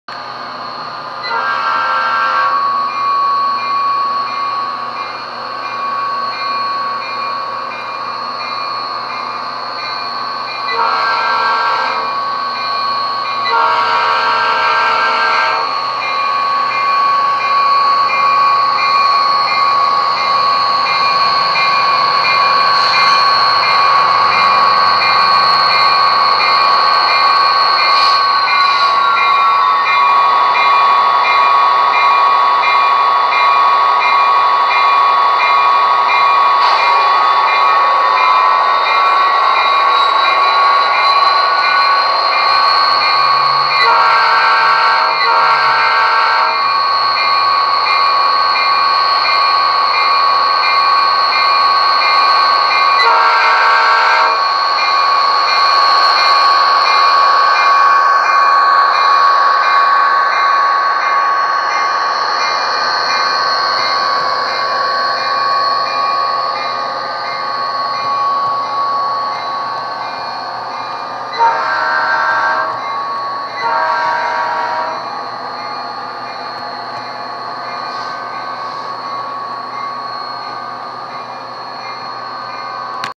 Rapido Amtrak F40PH stopping at the depot on the layout.
Awesome Amtrak F40PH model from Rapido trains.